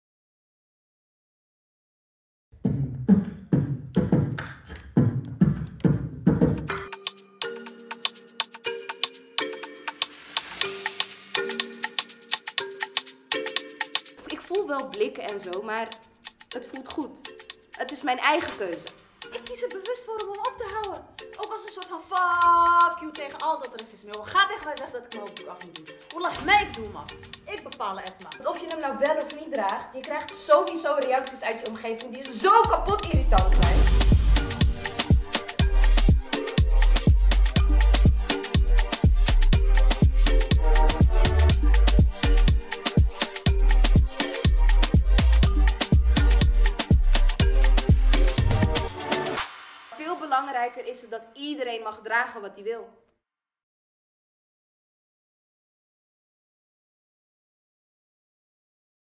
(W)RAP MY HIJAB - Trailer
(W)RAP MY HIJAB – Over wel of niet dragen en die andere vragen

Ik ben een stadsmens
Jong en getalenteerd
Rapper, bokser, danser, dromer
Ik hou van shoppen
Volg de mode
En tradities
Ik draag een hijab.
Die hoort bij mij
Maar definieert mij niet
Zie de hijab die mijn haren bedekt
En kijk ook naar mij
Wie ik ben

Voor (W)RAP MY HIJAB schrijven jonge meiden raps, spoken words en verhalen over het dragen van een hijab. Over hun ervaringen en keuzes. De vragen die ze vaak krijgen. Maar vooral die hen juist niet gesteld worden.

Regisseuse Khadija Massaoudi en dramaturge Saar Vandenberghe verwerkten deze persoonlijke verhalen in een muzikale performance met actrices Nora Akachar (Trauma’s van Nora) en Asma el Mouden (DEGASTEN). Geertje Geurtsen verzorgt de vormgeving en Ritzah Statia (Best of Fringe 2021) gaf bewegingsadvies. Na elke voorstelling praten Nora en Asma verder met het publiek.

Credits:

Regie: Khadija Massaoudi | Dramaturgie: Saar Vandenberghe | Spel: Nora Akachar en Asma el Mouden | Tekst: Jonge meiden uit Amsterdam Nieuw-West, Oost en Zuid | Bewegingsadviezen: Ritzah Statia | Muziek: Donisio Loswijk en Amjad Dwaer | Muzikaal advies: Abdelhadi Baaddi | Scenografie: Geertje Geurtsen | Productie: Raoul Petersen | Ontwikkeling, concept en educatie: Studio 52nd | Beeld: Ingrid Jongens | Scènefotografie: Robert van der Ree | Inhoudelijk advies: S.P.E.A.K., Leticia Nieto, Hanneke Felten (Movisie, KIS)